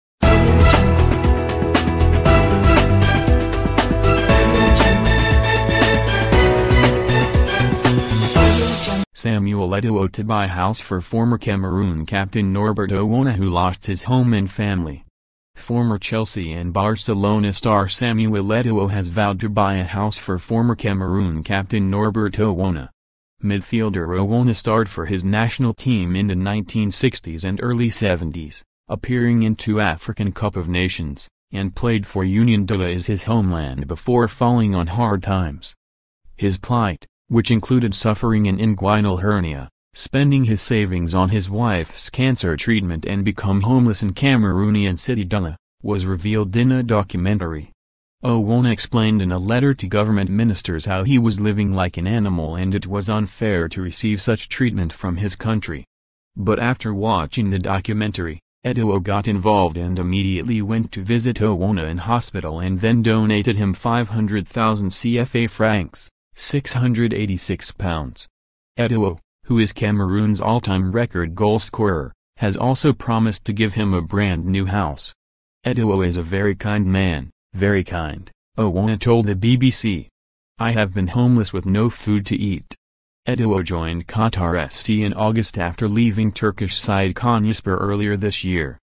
Samuel Eto'o to buy house for former Cameroon captain Norbert Owona who lost his home and
Samuel Eto'o to buy house for former Cameroon captain Norbert Owona who lost his home and family. Former Chelsea and Barcelona star Samuel Eto'o has vowed to buy a house for former Cameroon captain Norbert Owona.

Midfielder Owona starred for his national team in the 1960s and early 70s, appearing in two African Cup of Nations, and played for Union Douala is his homeland before falling on hard times.

His plight, which included suffering an inguinal hernia, spending his savings on his wife'sSamuel Eto'o,Cameroon football team